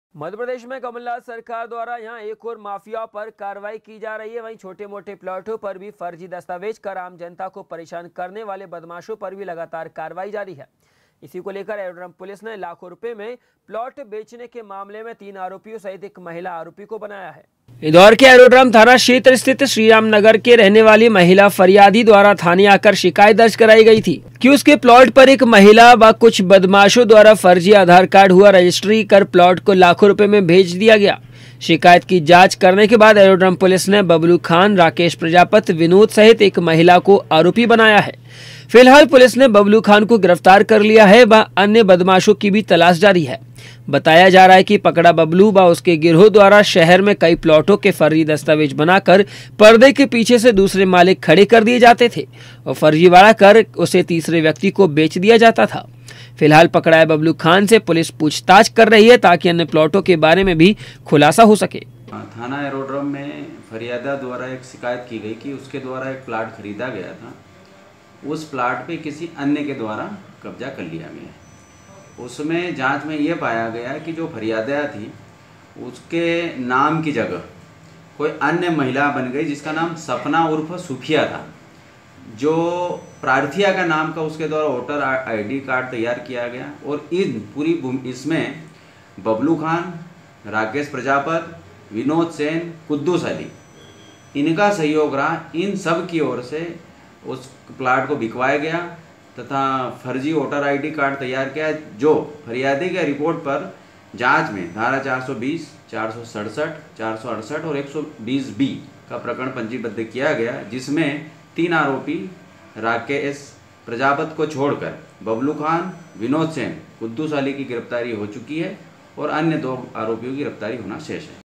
INDORE भूमाफियाओं पर प्रशासन की कार्रवाई
मध्यप्रदेश में कमलनाथ सरकार द्वारा जहां एक ओर माफियाओं पर कार्रवाई की जा रही है,,, वहीँ  छोटे-मोटे प्लाटों पर भी फर्जी दस्तावेज कर आम जनता को परेशान करने वाले बदमाशों पर भी लगातार कार्रवाई जारी है ,,,, इसी को लेकर एरोड्रम पुलिस ने  लाखों रुपए में प्लाट बेचने के मामले में तीन आरोपियों सहित एक महिला आरोपी को बनाया है ।